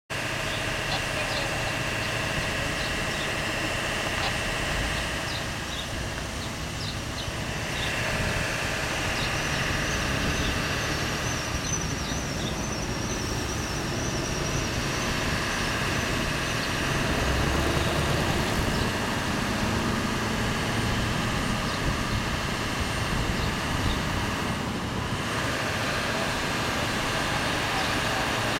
Fairhaven, MA Just another Fender Bender
MVA Sconticutneck and Jerusalem Rd No Injuries, Just happened to go by on GMC Mountain Bike with camera gear on board